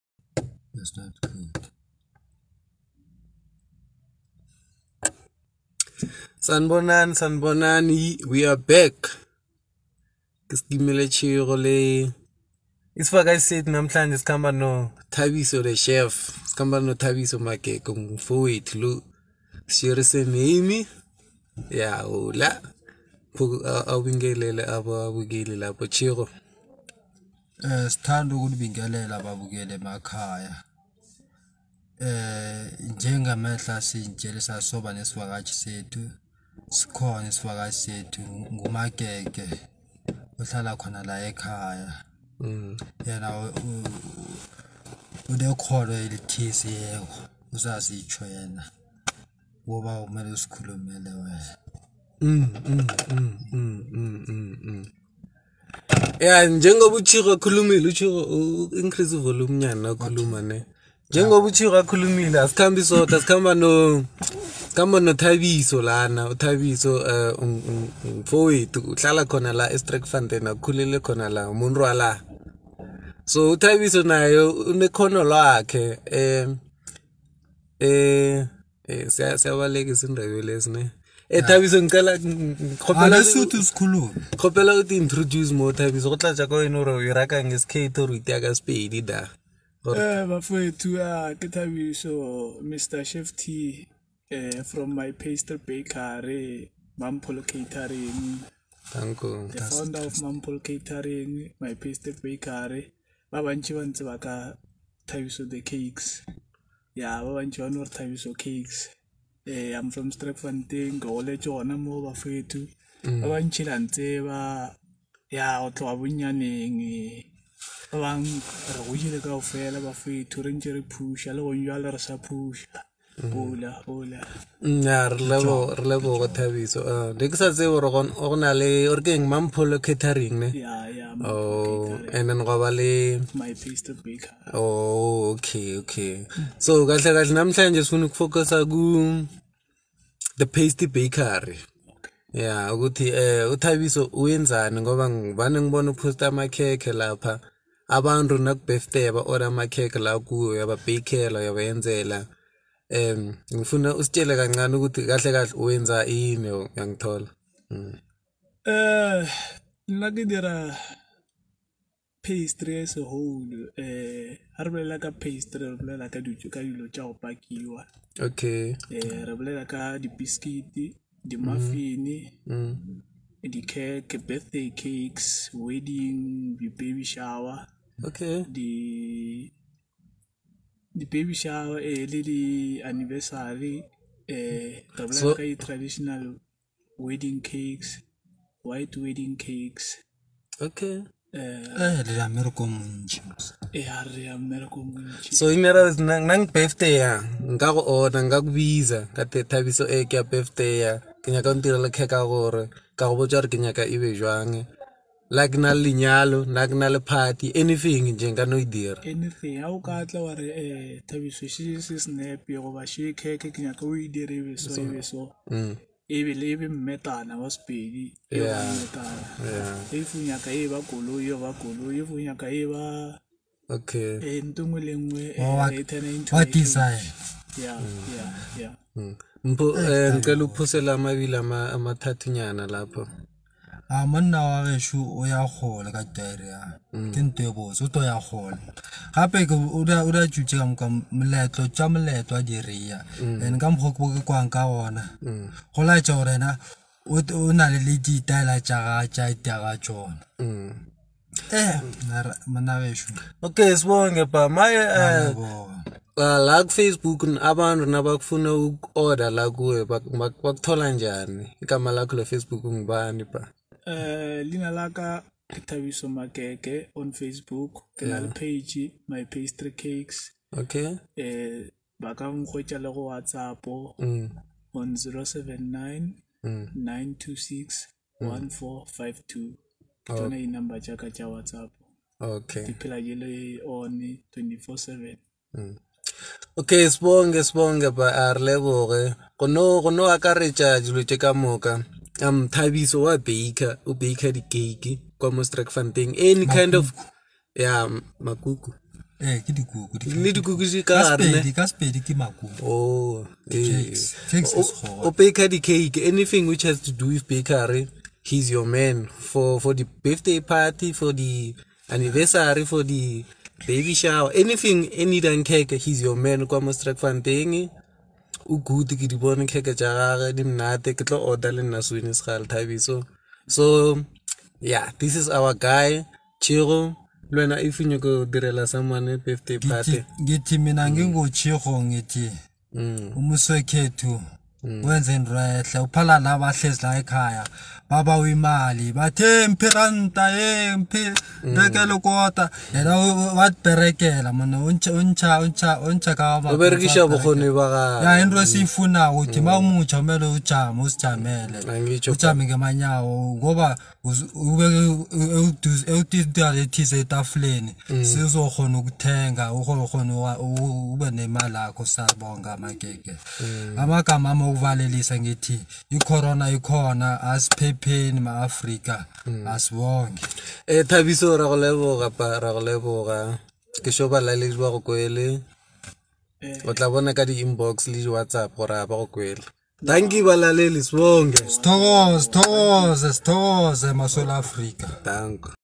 Thabiso's cakes - Baking Business - Mpho&Dave TalkShow Episode 5 -
Thabiso taking us through his pastry cakes business